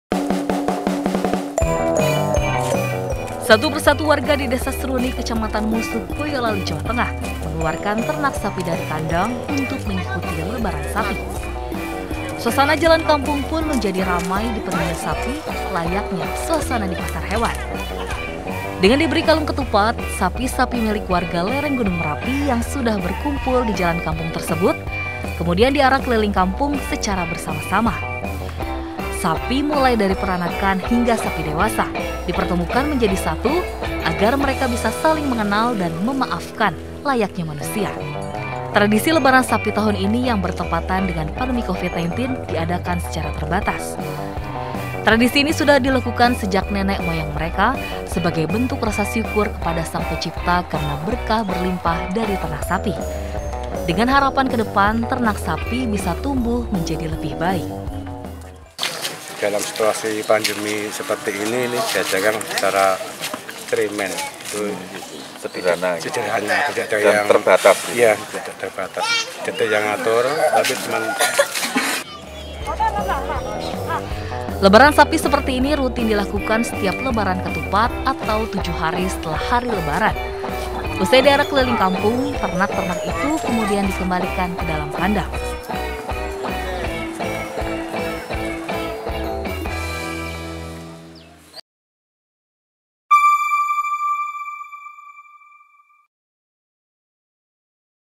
Lebaran Sapi, Sapi Berkalung Ketupat Dikumpulkan dan Saling Memaafkan
Tradisi syawalan yang unik, dilakukan warga lereng Gunung Merapi, di Boyolali, Jawa Tengah, yaitu  lebaran sapi.

Tradisi lebaran sapi dilakukan dengan mengarak sapi keliling kampung dan diberi kalung ketupat.